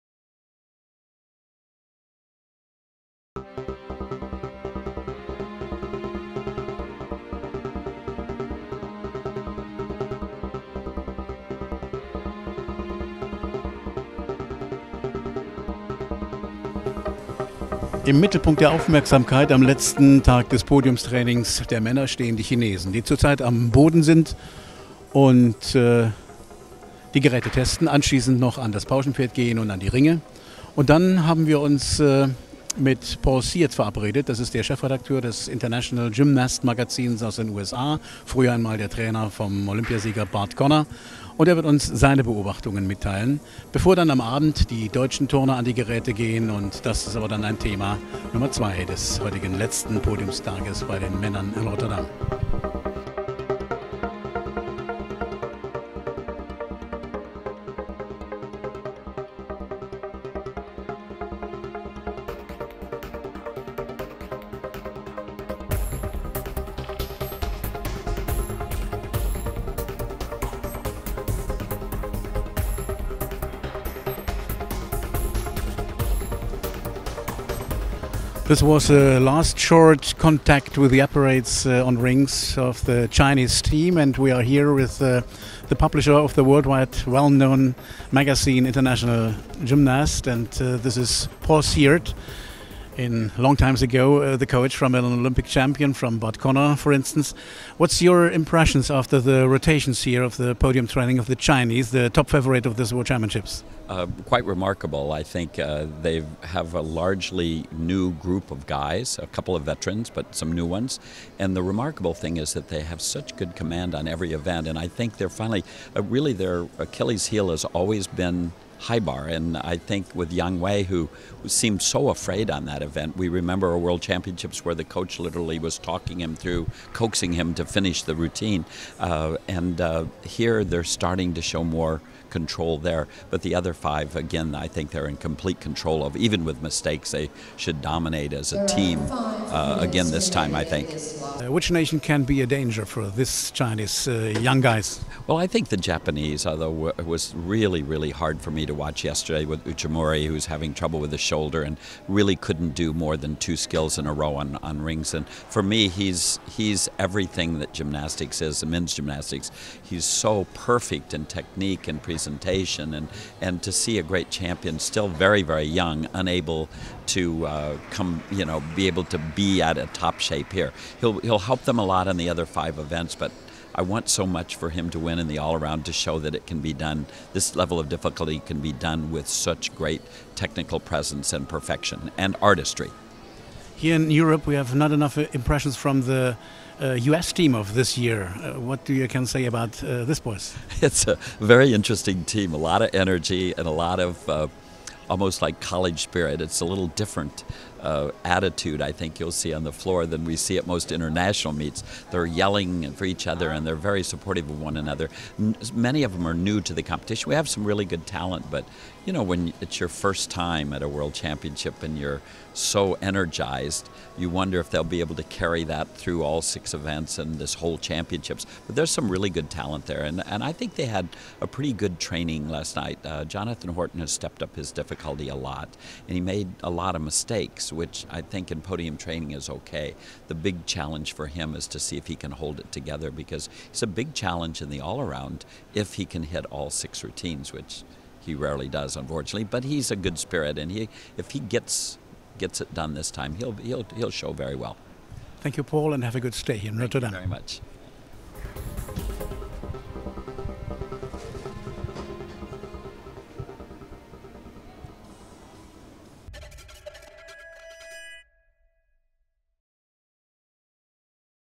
GYMmedia exclusive: Paul ZIERT (USA)
* Rotterdam, 2010, Oct 15
The Publisher of the US-Magazin "International Gymnast", Mr. Paul ZIERT, observed the podium training of the Chinese men's team and spoke in an interview with the chief editor of GYMmedia INTERNATIONAL, Eckhard HERHOLZ.